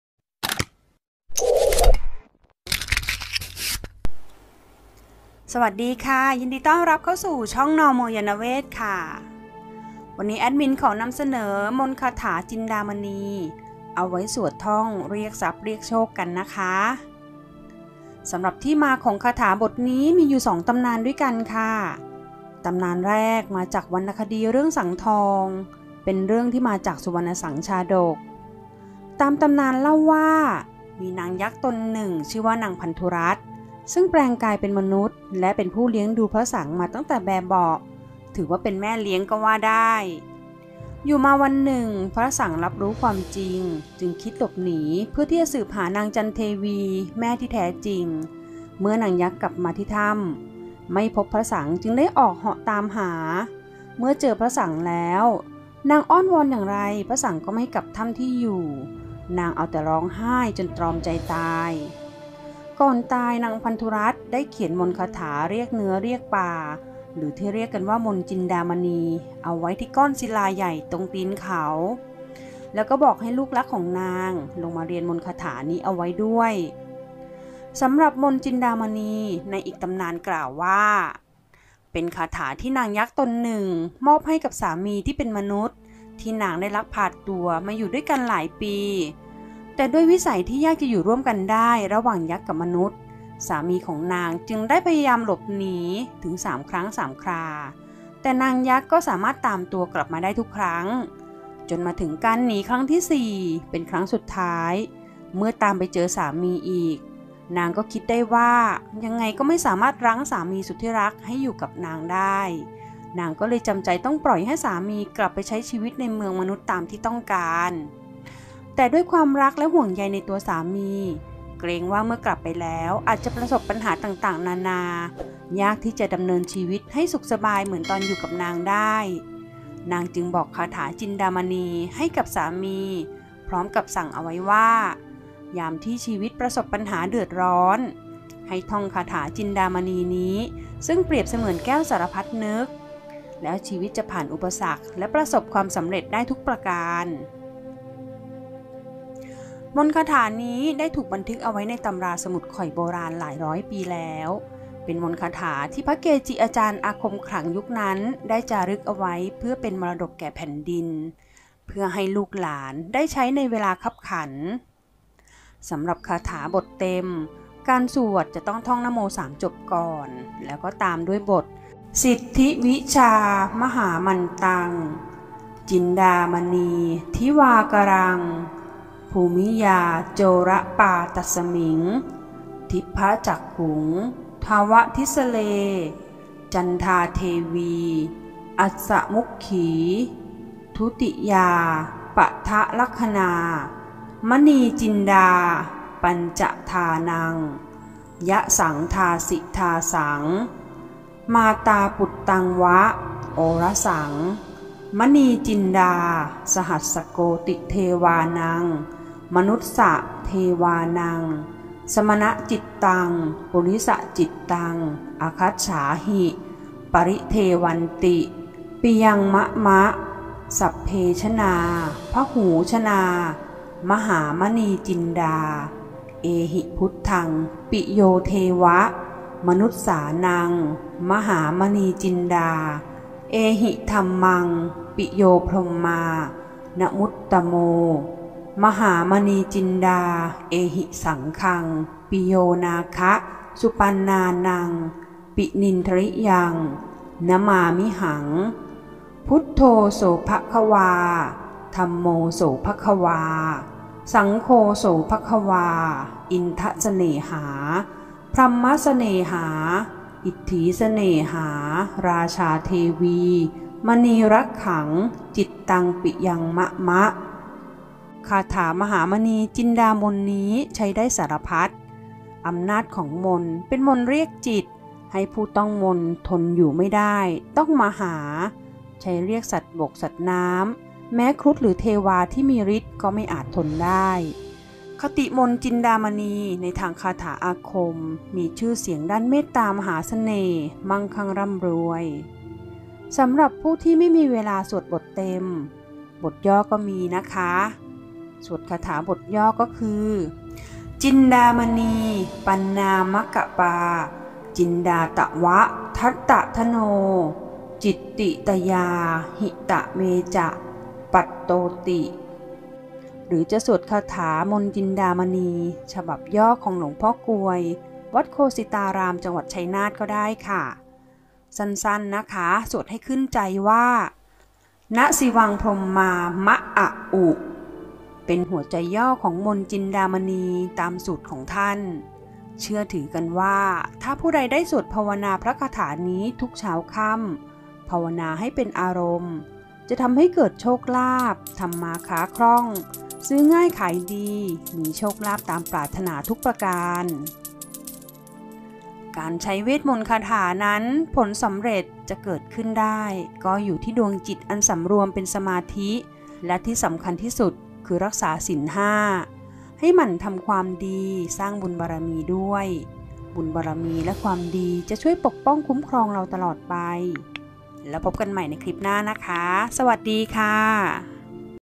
มนต์คาถาจินดามณีเรียกทรัพย์ | ตำนานมนต์จินดามณี
สำหรับคาถาบทเต็มการสวดต้องท่องนะโม 3 จบก่อนแล้วตามด้วย
สิทธิวิชา         มหามันตัง      จินดามณี         ทิวากะรัง
ภูมิยาโจระปาตัสสะมิง   ทิพพะจักขุง      ถาวะทิสสะเร      จันทาเทวี         อัสสะมุกขี
ทุติยาปะทะลักขะณา   มณีจินดา         ปัญจะทานัง      ยะสังทาสิทาสัง      มาตาปุตตังวะ
โอระสังฯ      มณีจินดา         สะหัสสะโกฏิเทวานัง   มะนุสสะ         เทวานัง
สะมะณะจิตตัง      ปุริสะจิตตัง      อาคัจฉาหิ      ปะริเทวันติ      ปิยังมะมะ
สัพเพชะนา      พะหูชะนา      มหามณีจินดา      เอหิพุทธัง      ปิโยเทวะ
มะนุสสานัง      มหามณีจินดา      เอหิธัมมัง         ปิโยพรหมมา      นะมุตตะโม
มหามณีจินดา      เอหิสังฆัง      ปิโยนาคะ      สุปันนานัง      ปินินทริยัง
นะมามิหัง      พุทโธโสภะคะวา      ธัมโมโสภะคะวา      สังโฆโสภะคะวา      อินทะเสน่หา
พรัหมมะเสน่หา      อิตถีเสน่หา      ราชาเทวี         มณีรักขัง         จิตตัง

คาถาบทย่อคือ
"จินดามณี ปันนามะกะปา จินดาตะวะ ทัตตะทโน จิตติตะยา หิตะเมจะ ปัตโตติ
หรือจะสวดคาถามนต์จินดามณี"
ฉบับย่อของ หลวงพ่อกวย วัดโฆสิตาราม จ.ชัยนาท
  “นะ สิวัง พรหมา มะอะอุ”